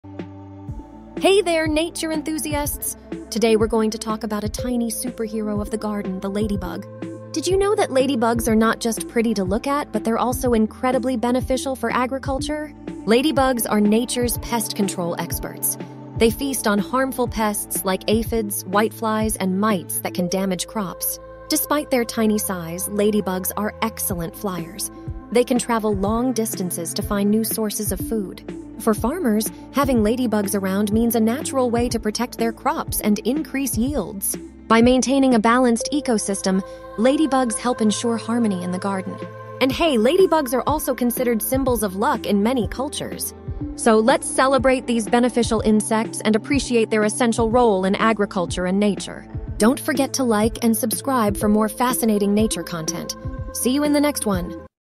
Nature's Tiny Superheroes: The Incredible Ladybugs!
Discover the amazing world of ladybugs, nature's guardians of the garden! Learn how these tiny superheroes help protect crops, maintain balance, and bring luck to your plants. Join us to uncover the secrets of these beneficial insects and their vital role in agriculture and nature. 🐞🌱